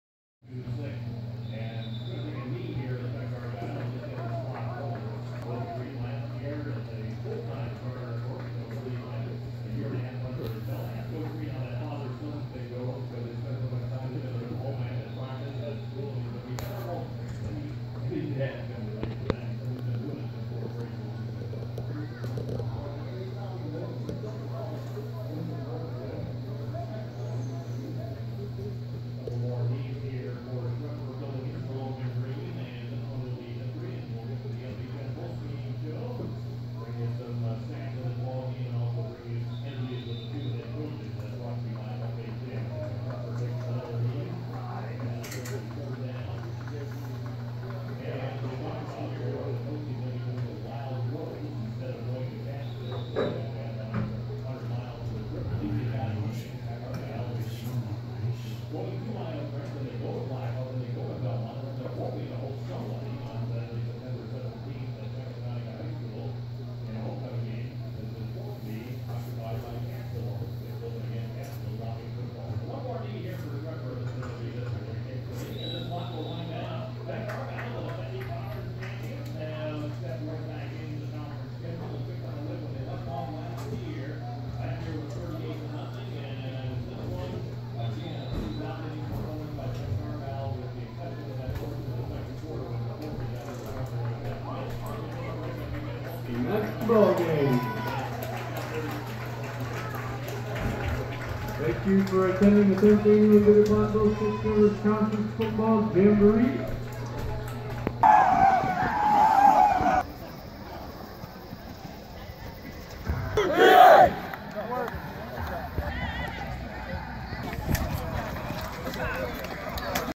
Potosi vs. Pecatonica/Argyle - Final Two Minutes
Week 1 - Saturday.

The fifth and final game of the annual Six Rivers Conference Jamboree concludes with the defending conference champion Vikings notching a 36-6 victory.